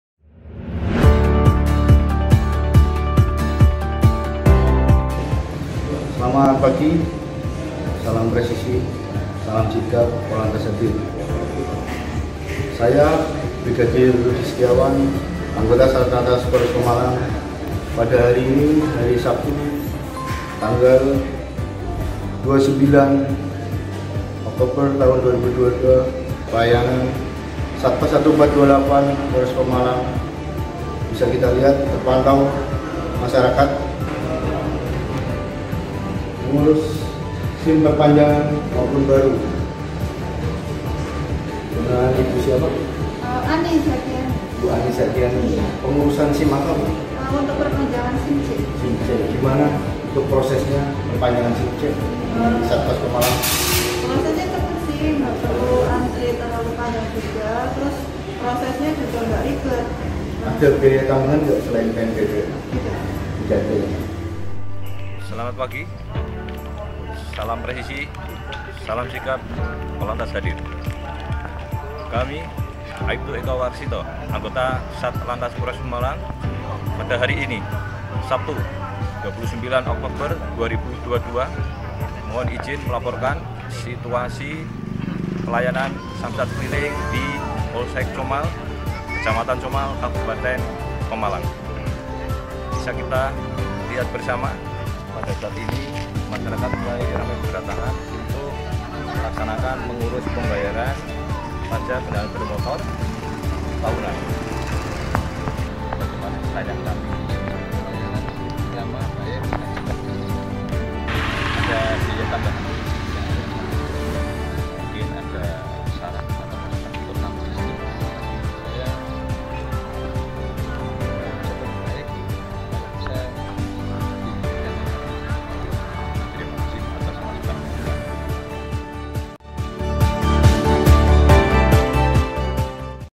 Testimoni Masyarakat Untuk Pelayanan di Satpas dan Samsat Pemalang
Live Report serta Testimoni Masyarakat Untuk Pelayanan di Satpas dan Samsat Pemalang.